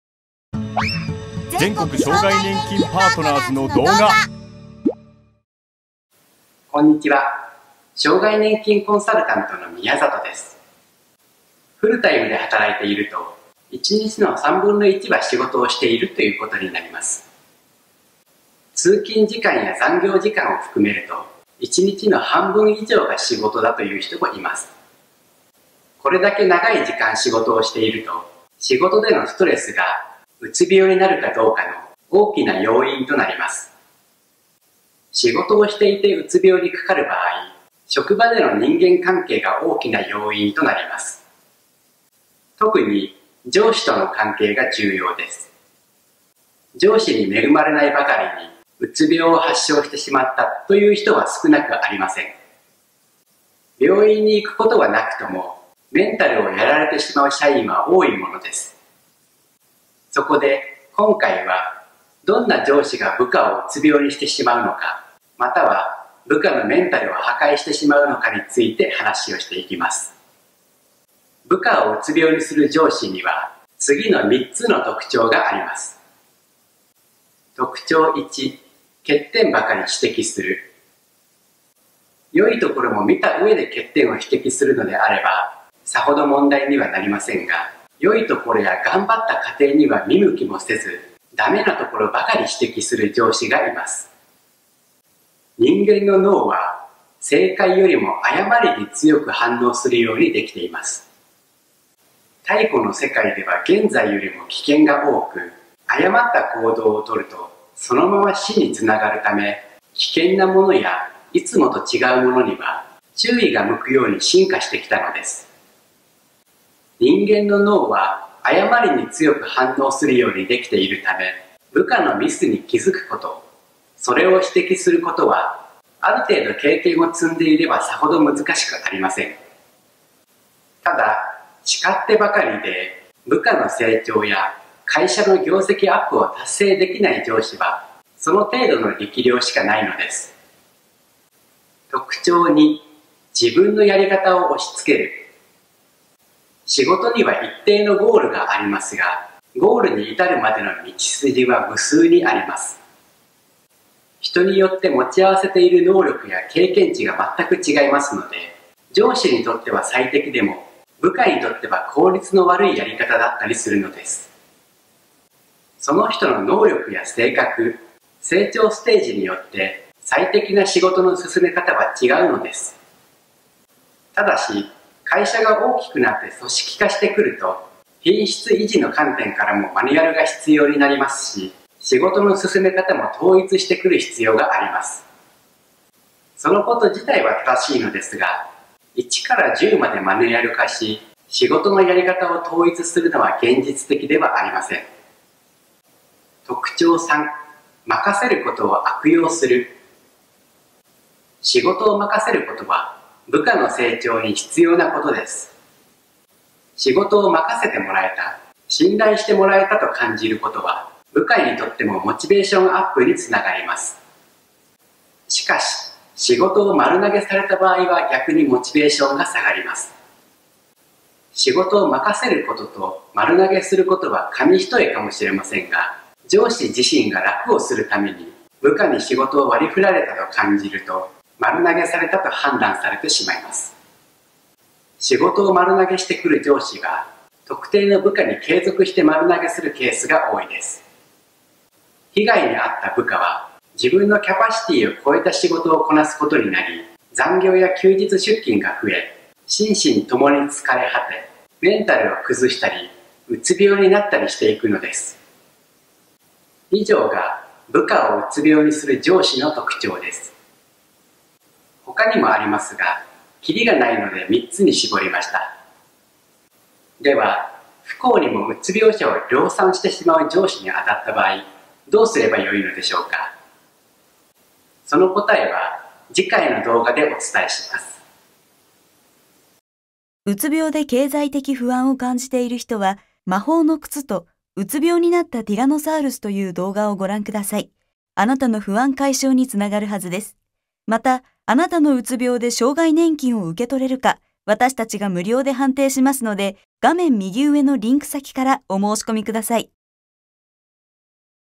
部下をうつ病にする上司の3つの特徴【社会保険労務士事務所全国障害年金パートナーズ】
このビデオでは 部下をうつ病にする上司の3つの特徴をお伝えします。
無料レポートのプレゼントや、障害年金の無料判定も行っています！